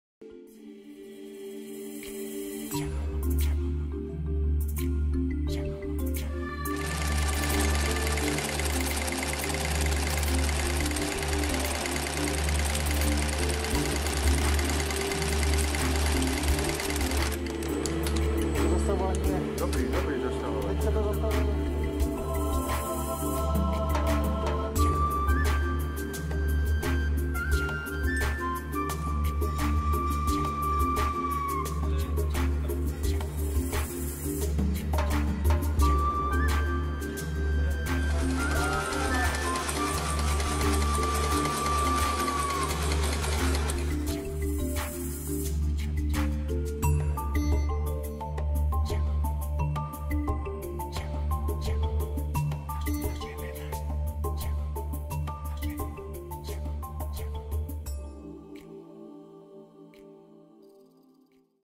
Sole stitching machine - 03028 P11 Traco Machinery
SOLE STITCHING SHOESTICKING MACHINE made by Traco machinery tested by Bata shoemaker.
Shoemaking  machines and equipments producer.